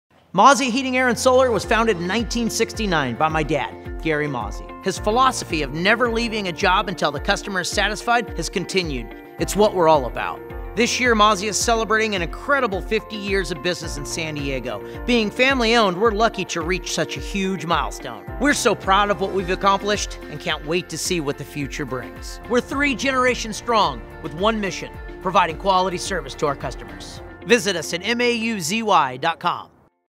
Mauzy Heating, Air & Solar 50th Anniversary
BE-Media Films produced this 50th anniversary spot for our friends at Mauzy.